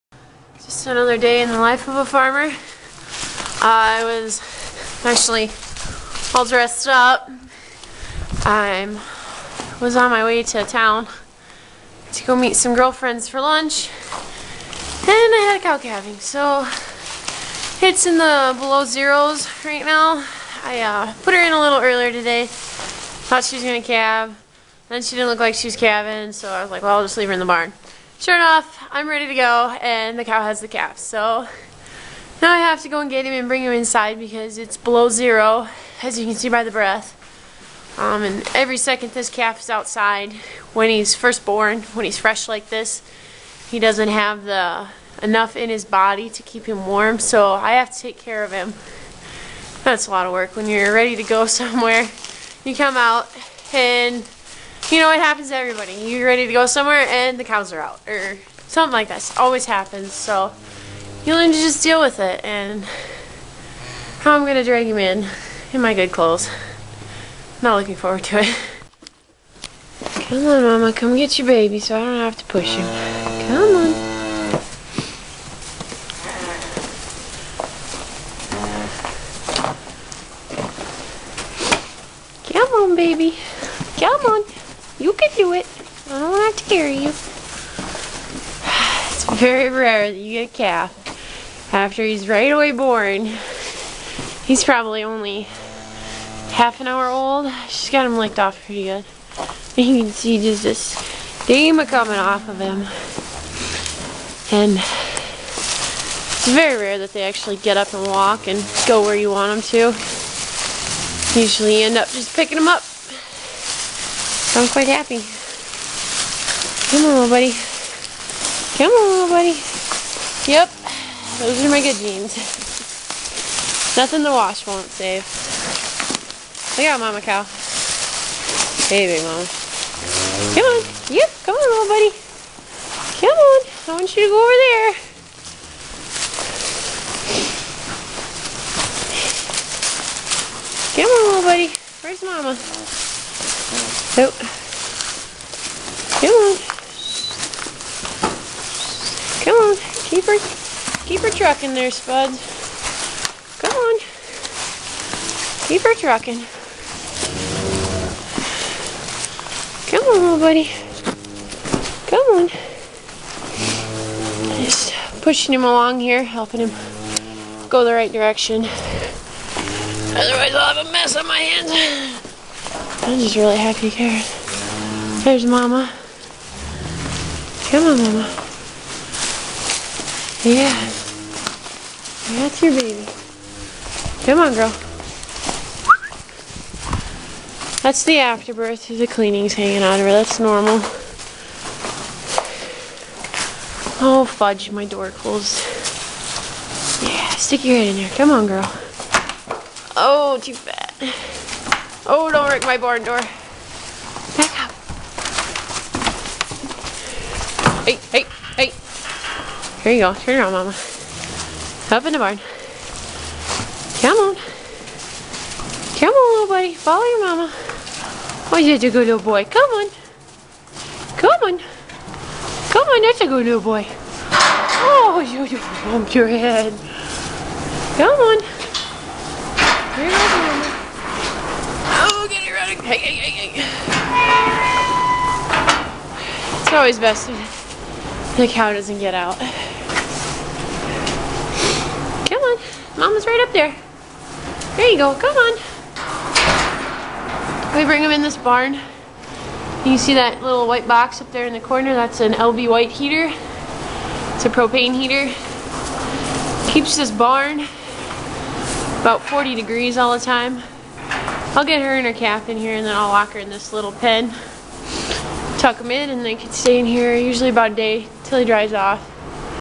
lipstick, lunch, and calving
If you have cows than you can relate to this video. It never fails, 9 times out fo 10 when I have plans and am about to leave the cows have other plans. I have come to live with it and my friends are used to the fact that my cattle come first no matter the occation.